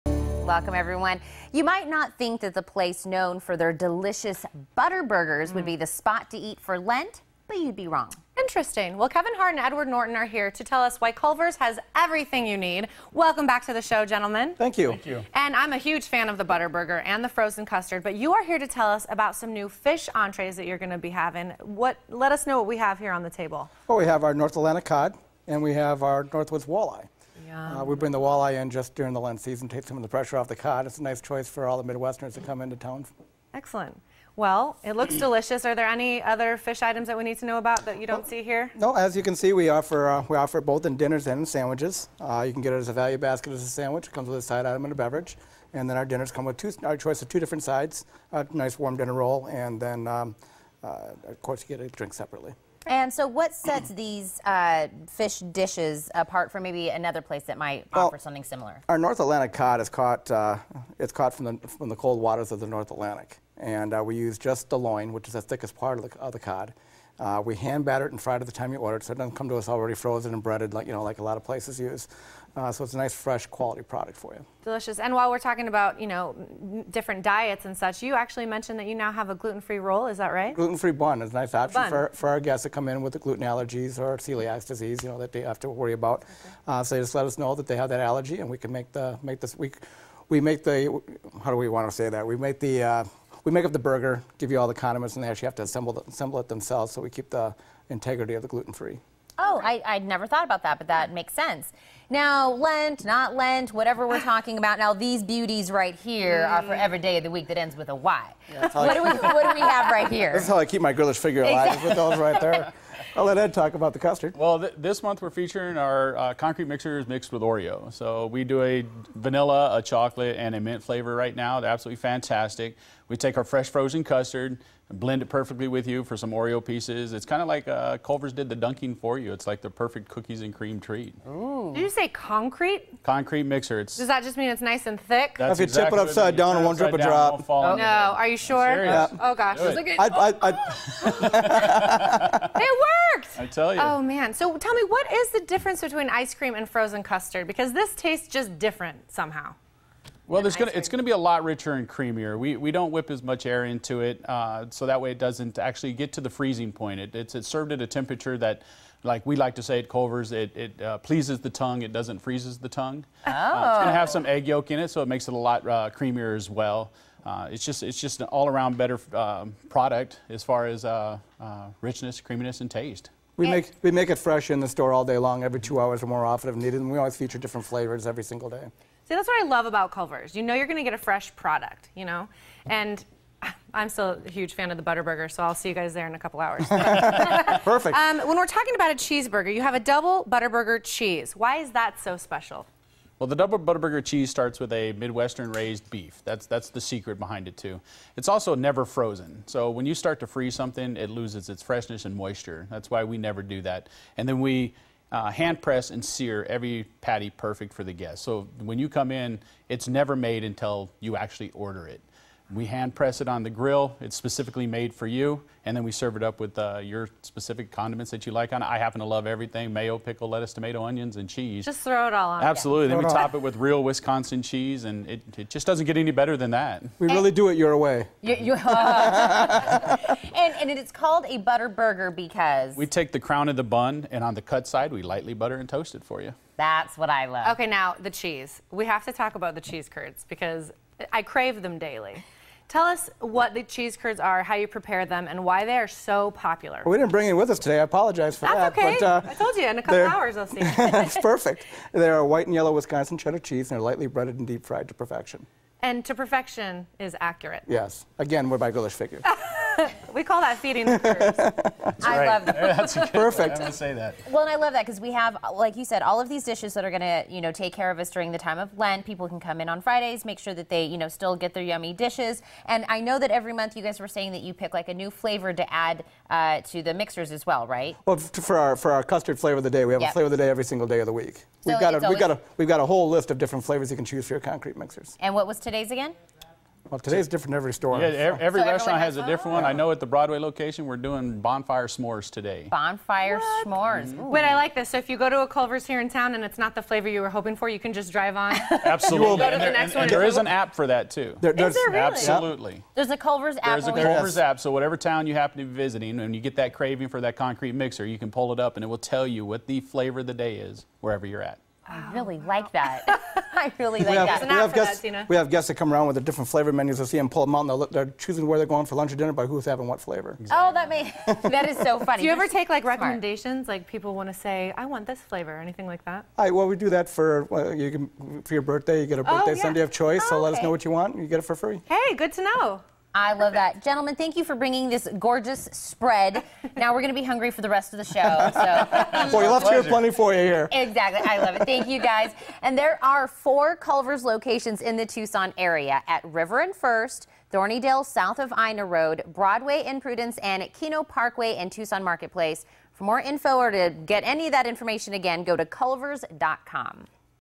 Culver's - North Atlantic Cod and other guest favorites
-Culver’s ButterBurgers and Fresh Frozen Custard
-North Atlantic Cod is a guest favorite all year long, but especially during Lent
-Concrete Mixers made with Oreo are a deliciously creamy treat
-Double ButterBurger Cheese, Onion Rings & Dr. Pepper are Culver’s classics